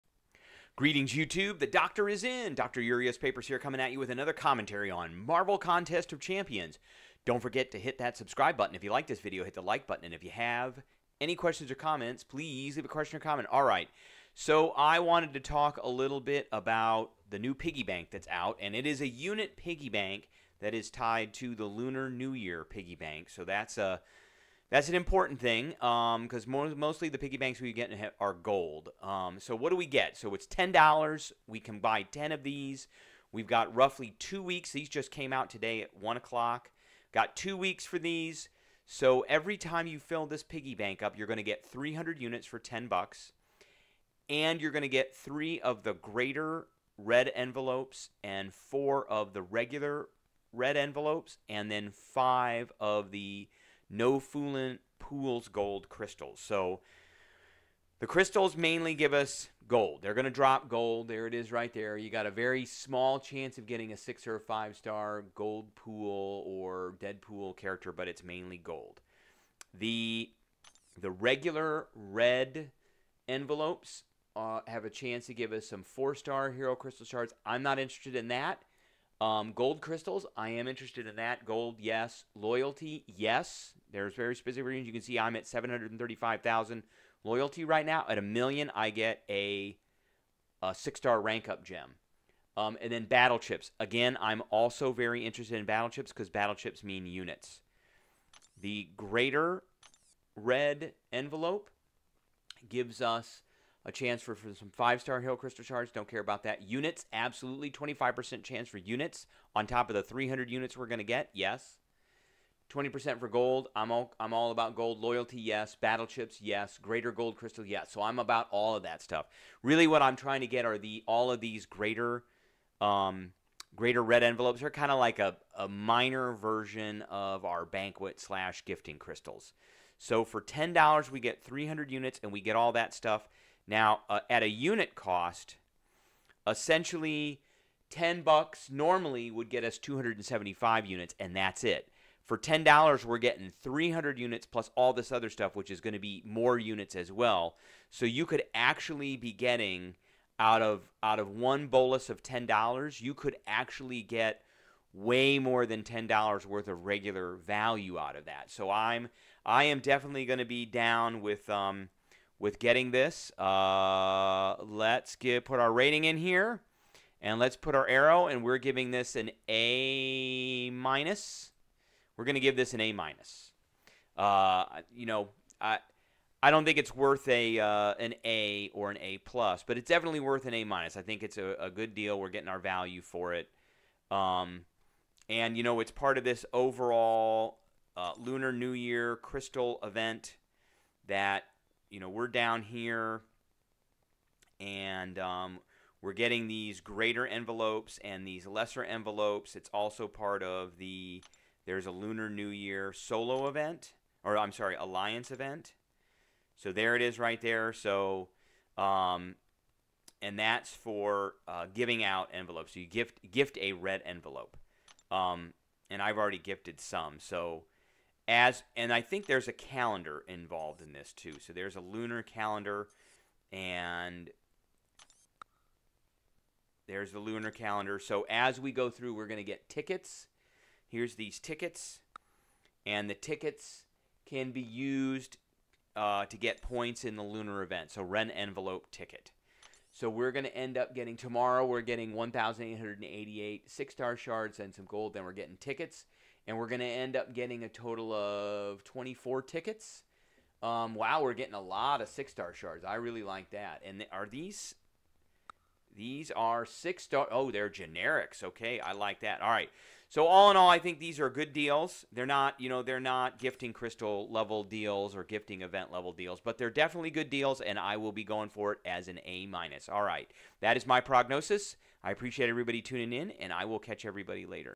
Doctors Prognosis of the 2023 MCOC Lunar New Year Event and Piggy Bank
In this video, we give our prognosis of the 2023 Lunar New Year event and Piggy Bank.